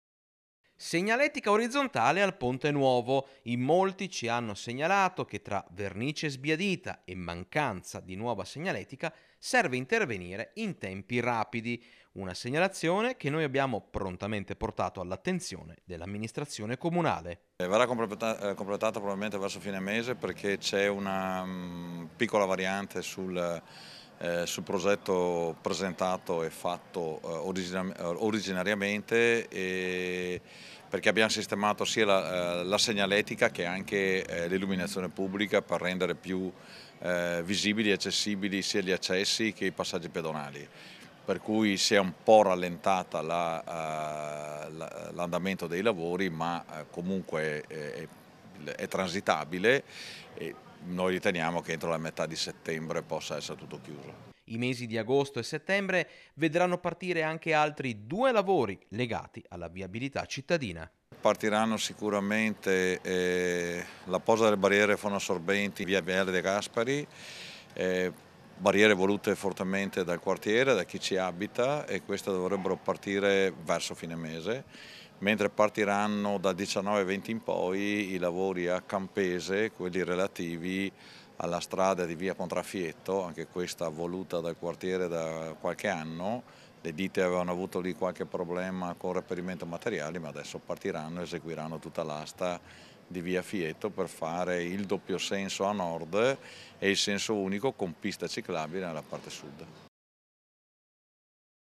SEGNALETICA ORIZZONTALE SUL PONTE NUOVO, ZONTA: «A BREVE COMPLETEREMO I LAVORI» | 06/08/2024
TG Bassano del Grappa 06/08/2024 - BASSANO DEL GRAPPA - Strisce pedonali sul Ponte Nuovo, presto verranno realizzate || Segnaletica orizzontale al Ponte Nuovo: in molti ci hanno segnalato che fra vernice sbiadita e mancanza di nuova segnaletica serve intervenire in tempi rapidi… Una segnalazione che noi abbiamo prontamente portato all’attenzione dell’Amministrazione comunale. - Intervistati ANDREA ZONTA (ASS. VIABILITA' - BASSANO) (Servizio di Alex Iuliano) - Segui Rete Veneta anche sul digitale terrestre! Visita il sito